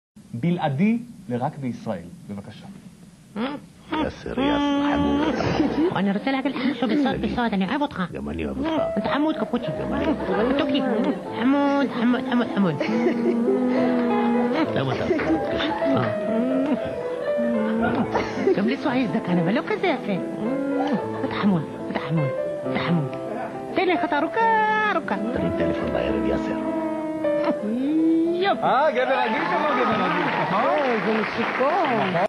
Yassir Arafat was homosexual
No wonder they say arafat died by sexual transferred disease...
This is taken from a comedy show on Israeli TV. Theres nothing much to translate